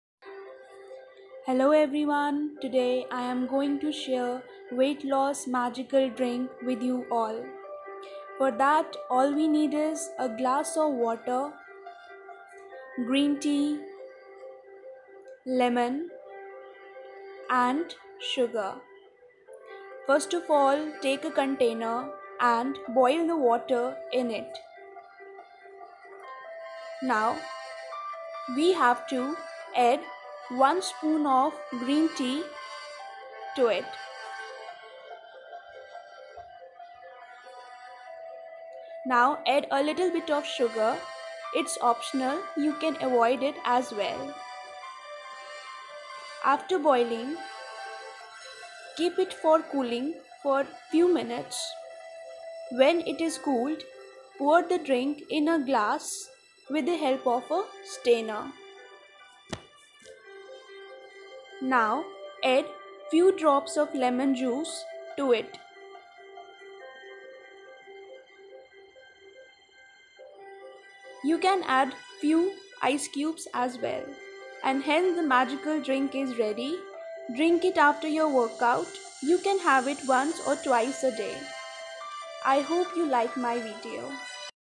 Weight loss magical drink after workout
Hello everyone.
I have shared a perfect drink for weightloss.
You should drink it regularly after workout.
You can have it empty stomach in the morning as well. But when taken empty stomach in the morning then sugar and ice cubes should be avoided.